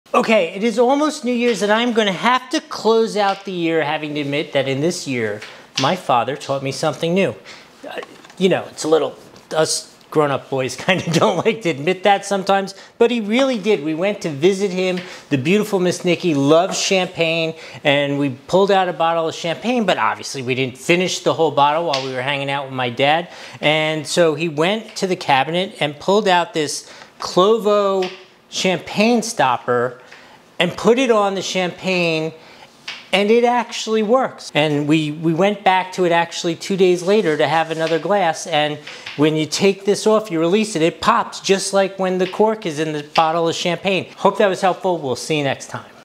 Explore the elegance and efficiency of the Kloveo Champagne Stoppers
You're also helping us create more awesome content for you commissionsearned. Click now and let's take your experience to the next level! (Amazon)

🍾✨ Discover the Magic of Kloveo! ✨🍾

Join us as we explore the elegance and efficiency of the Kloveo Champagne Stoppers - a must-have for every sparkling wine enthusiast! 🥂

Why Kloveo? Here's why:

🇮🇹 Made in Italy with Premium Quality
🍾 Patented Seal Technology - No more pressure pumps!
🥂 Perfect for Champagne, Prosecco, Cava, and more
💎 Professional Grade for the Ultimate Wine Experience
See it in action and understand why Kloveo is a game-changer in preserving the fizz and flavor of your favorite bubbly. 🎉